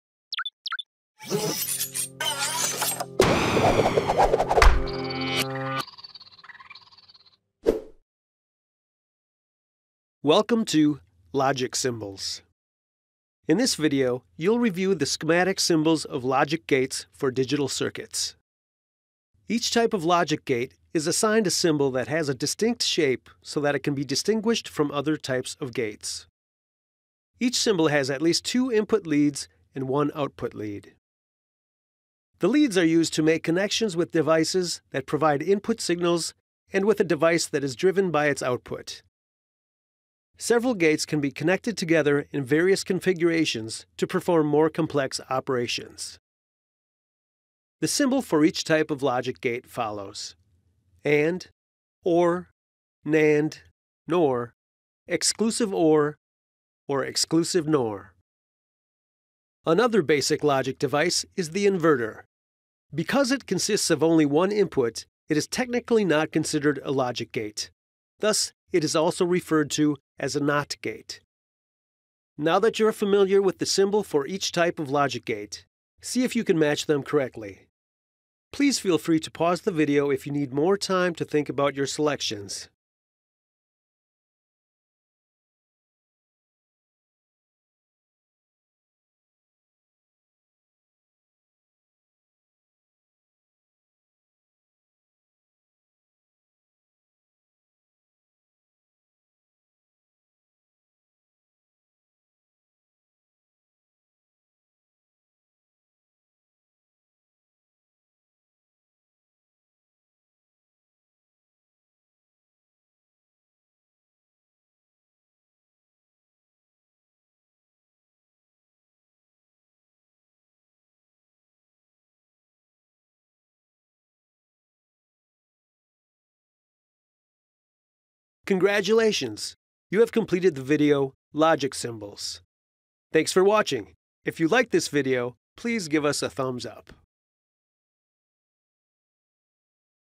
Logic Symbols
The schematic symbols of logic gates used in digital circuits are shown.

Credits:
    - Content Author: Terry Bartelt
    - Video Developer: Justin Stiper